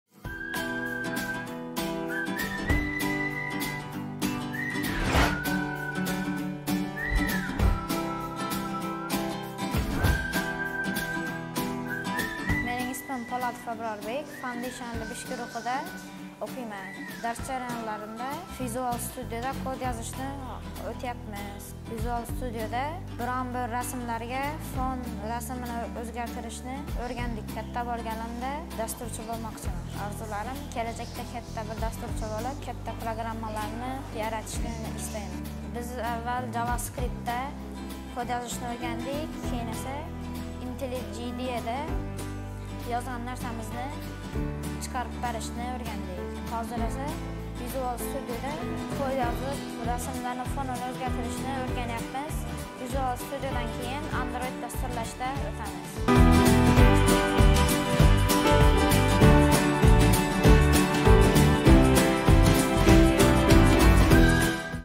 Bola boshidan dasturchi bo'lishi mumkin!
💁🏻‍♂️ Xuddi “Foundation” kursimiz o’quvchisi Abrorbek Po’latov kabi.

😎 Qahramonimiz dasturlashni o’rganishni veb-saytning tashqi ko’rinishini yasashdan boshladi. Keyinchalik esa, navbat Android dasturlashga!